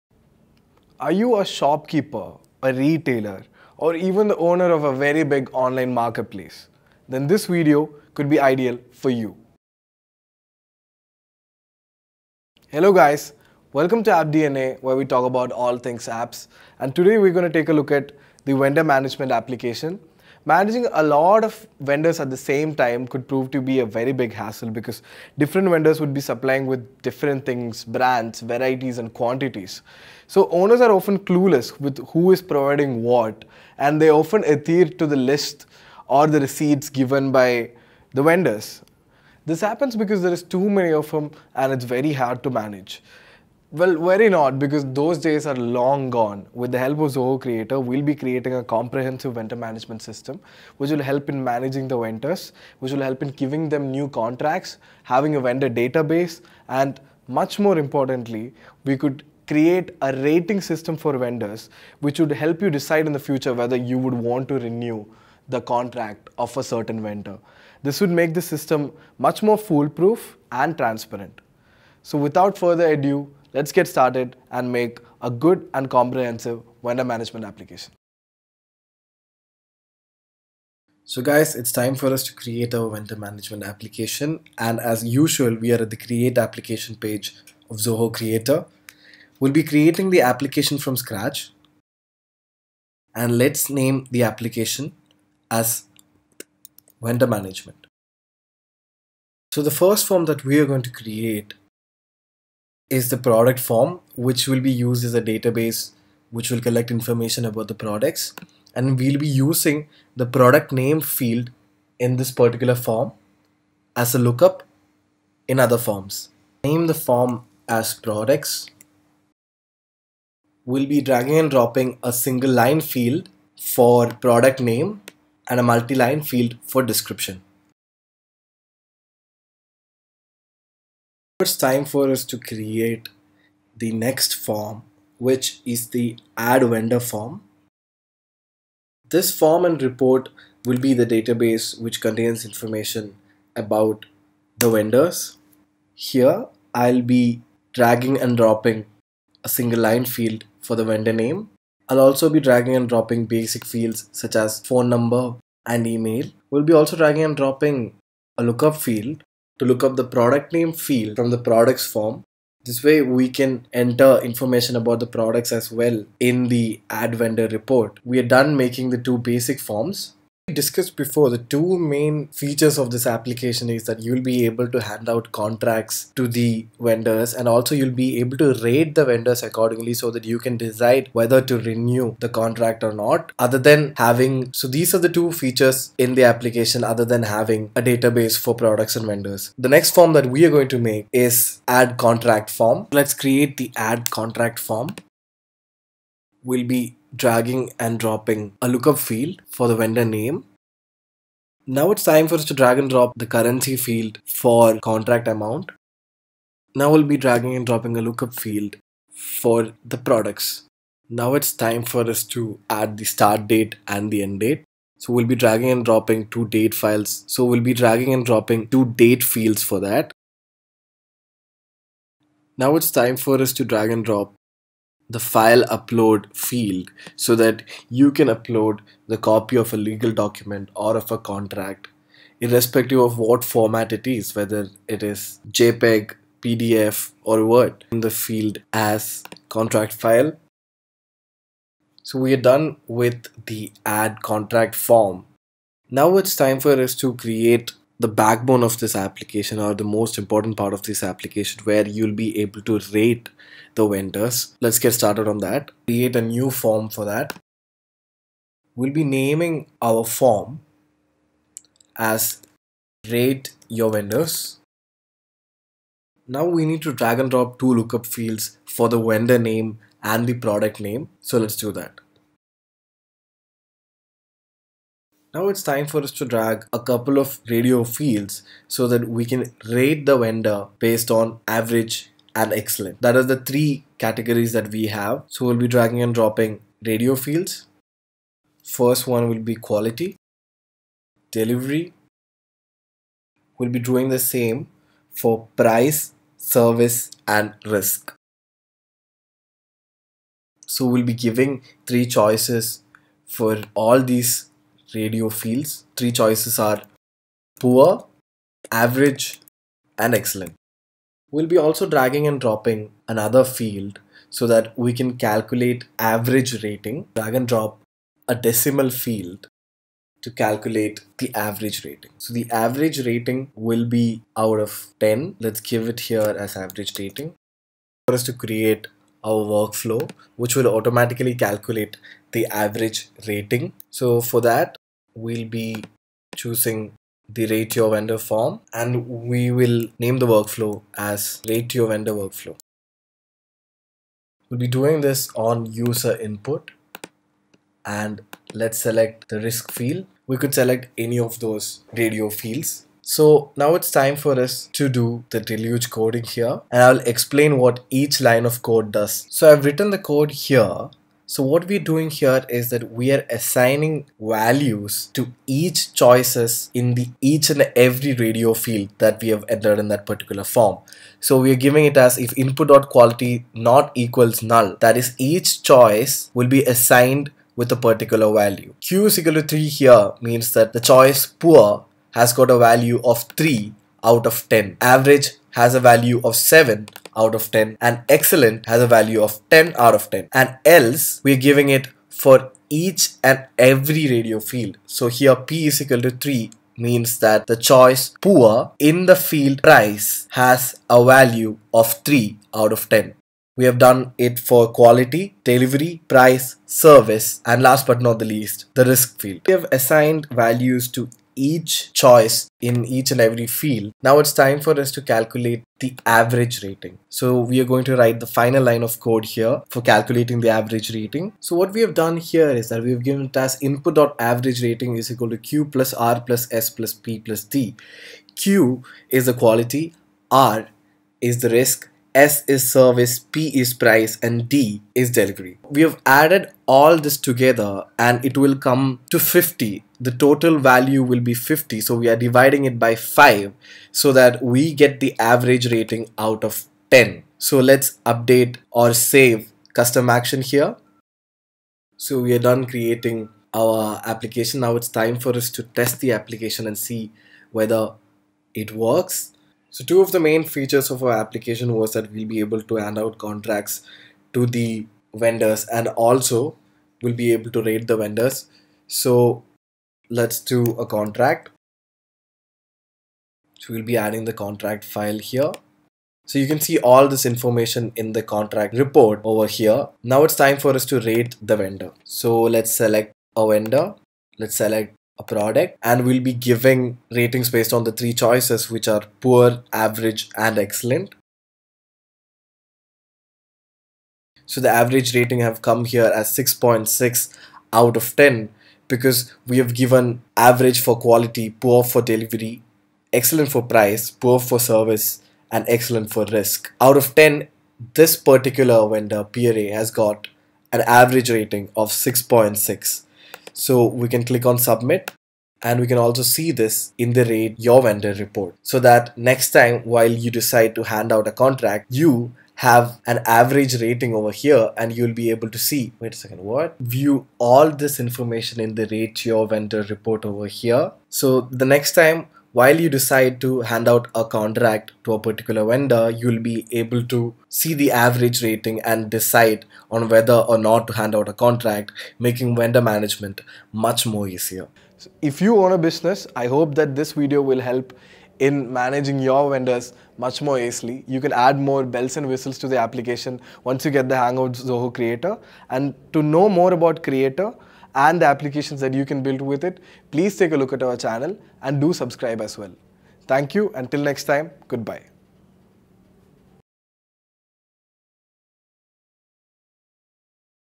Vendor Management Software | Build your own vendor management app | App DNA
Build your own custom vendor management app in 10 minutes with a low-code platform. You need not know coding for this!

Vendor management software is a discipline that allows companies to control expenses, improve service quality, and reduce risk to get more value from their vendors throughout the deal life cycle.

00:00 Introduction
01:58 Products form
02:20 Vendor form
03:19 Add contract form
04:25 Rate your vendor form